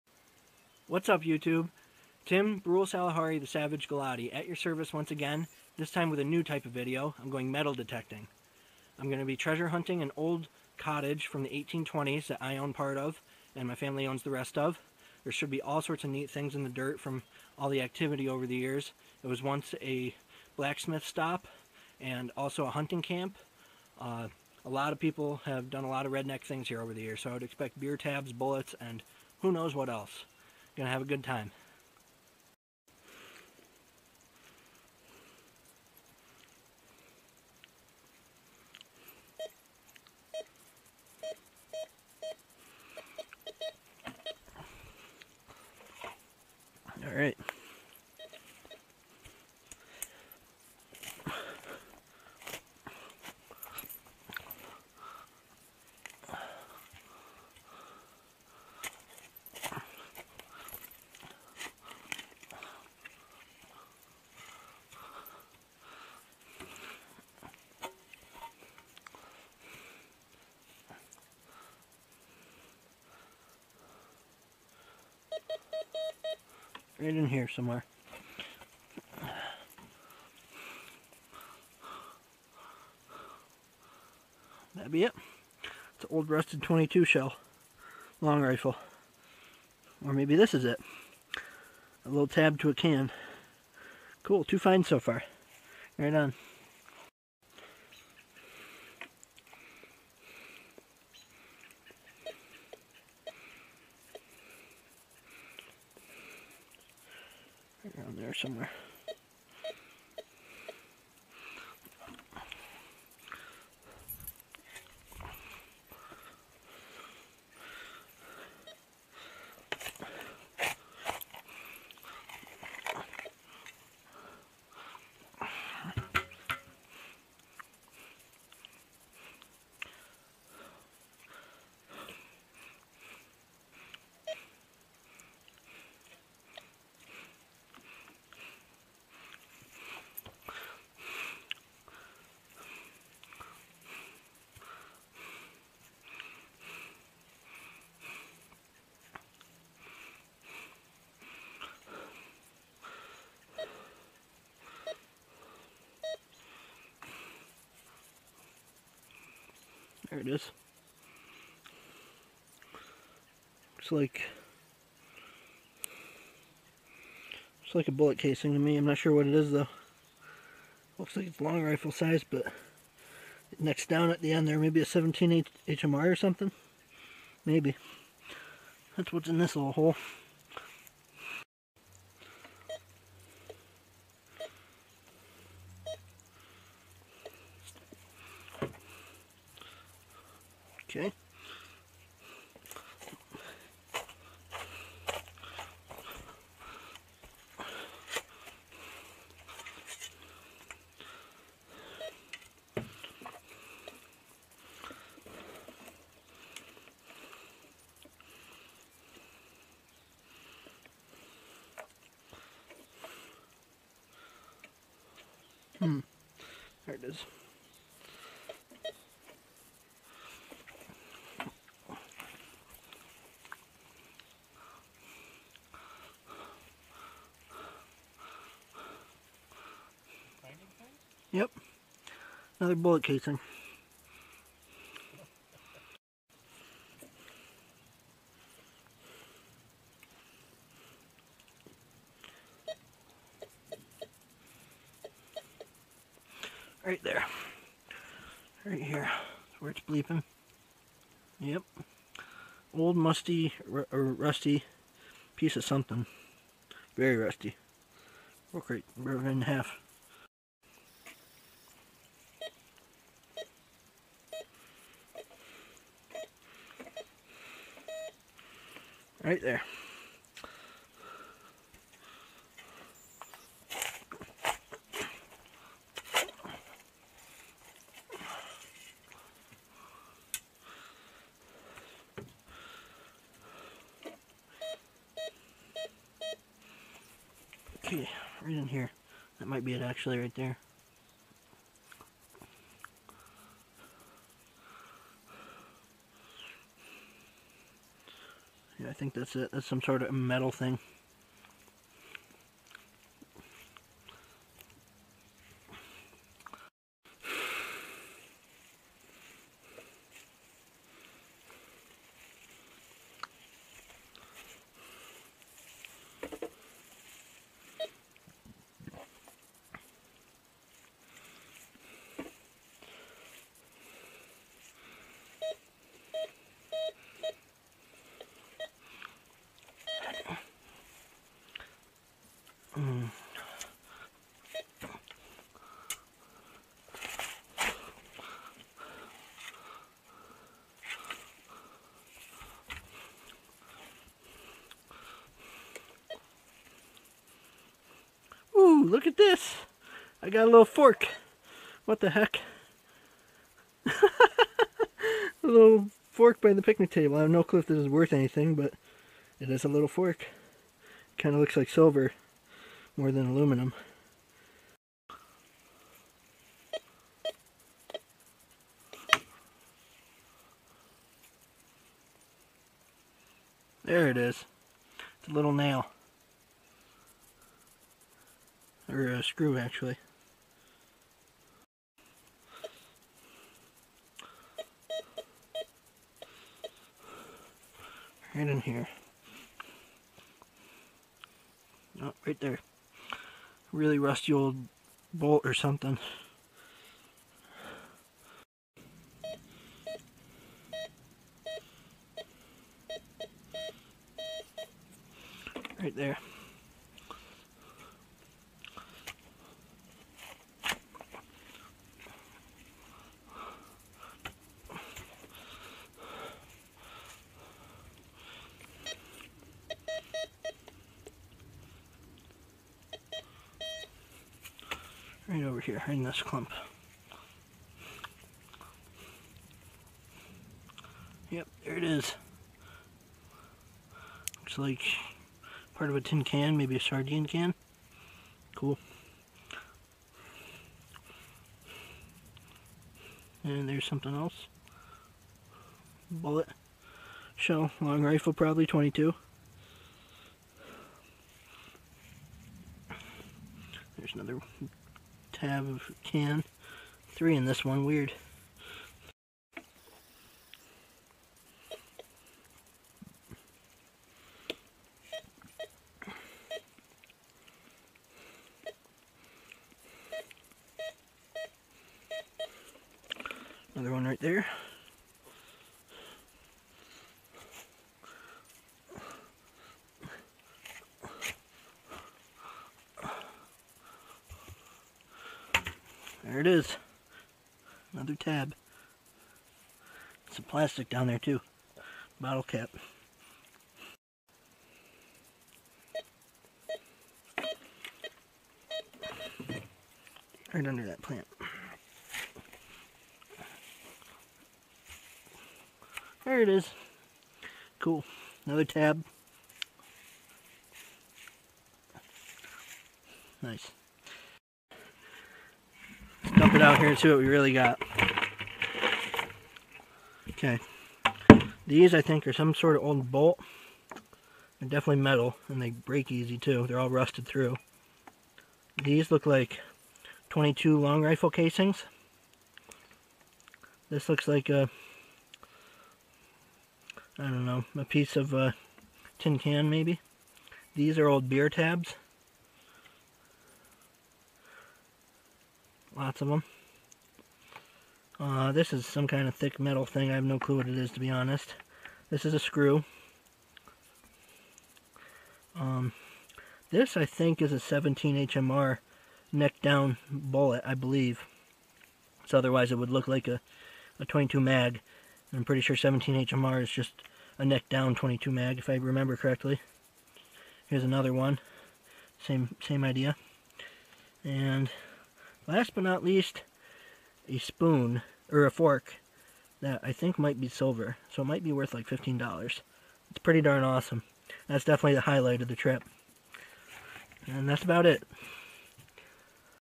Vlog #75: Metal Detecting at 1820s Cottage | Bullet Casings, Beer Tabs, and a Silver Fork! (P1)
This is my seventy fifth vlog (video blog) filmed in July, 2018. It shows an evening of metal detecting (treasure hunting) around the picnic area of an 1820s wooded upstate New York farmhouse that has served as a blacksmith shop and hunting camp for the last two centuries. During this session, I found beer tabs, bolts, a screw, bullet casings, and a baby silver fork.